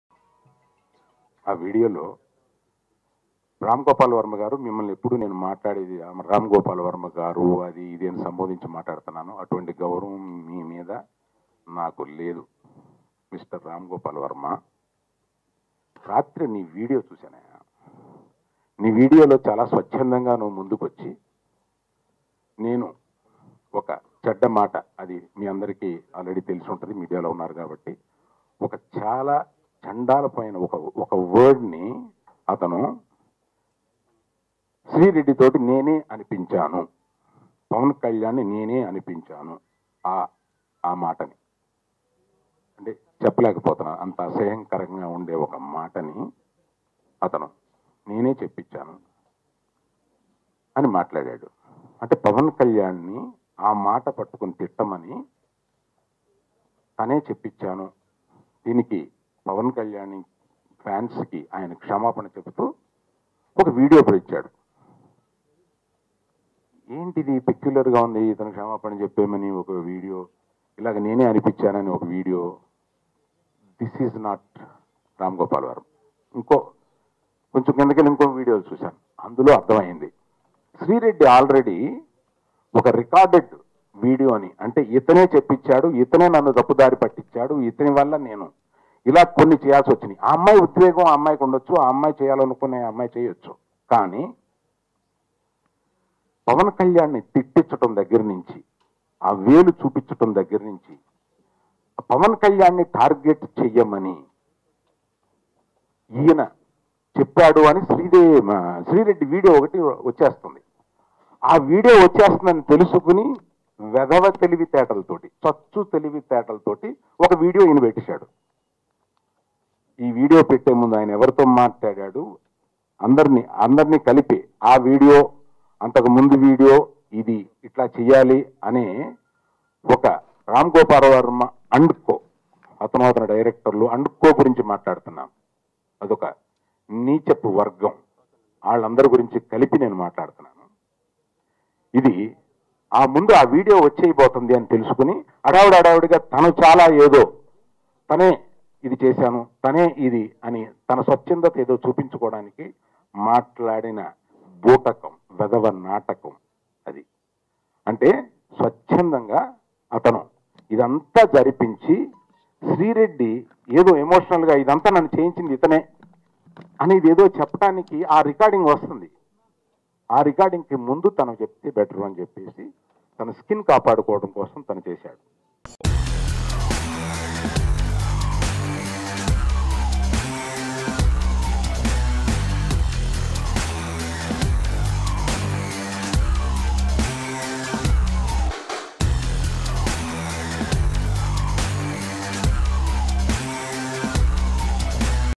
Allu Aravind Warning to RGV and his Group || Allu Aravind Press Meet || Sri Venkateswara Movies
Watch Allu Aravind Warning to RGV and his Group over Sri Reddy and Pawan Kalyan Issue.